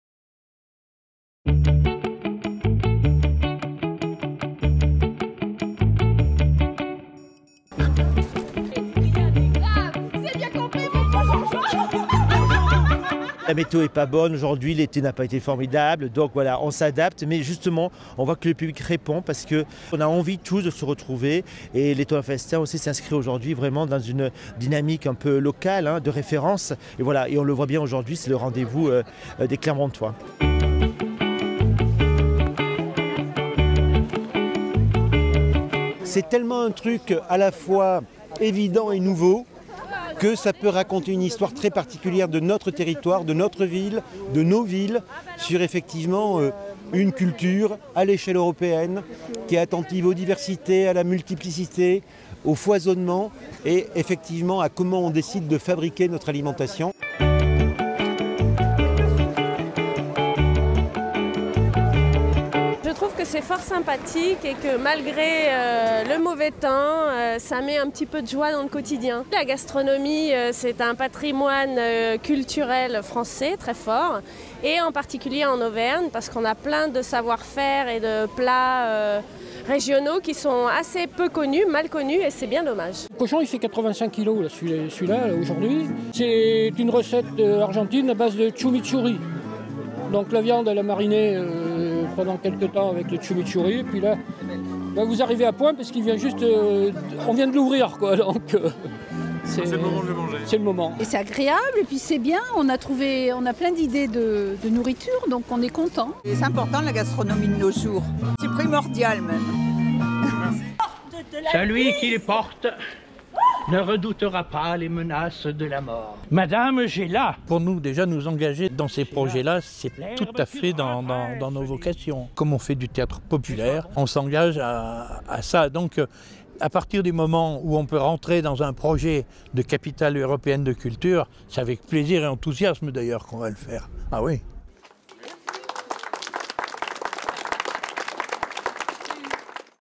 L'ÉTONNANT FESTIN/ BONS MANGEURS SOUS CIEL PLEUREUR
Malgré la pluie, l'Étonnant Festin a une fois encore réussi son pari, réunir autour du bien manger en toute convivialité.